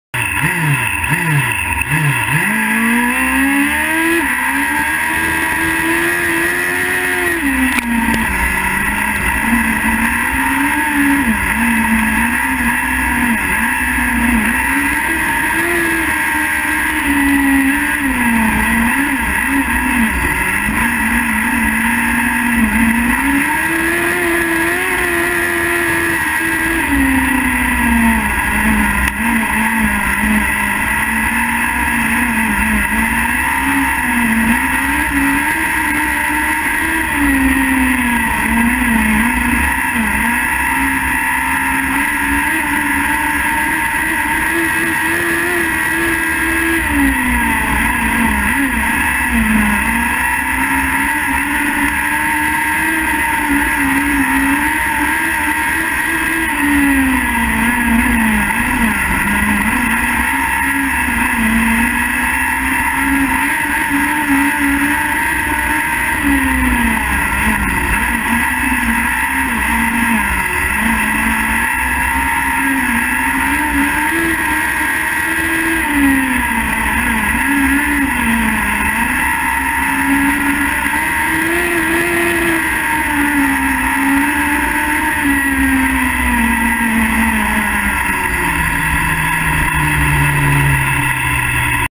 Heat 3 16 2 18 Palmerston North
The work of my Swinger to get me round the track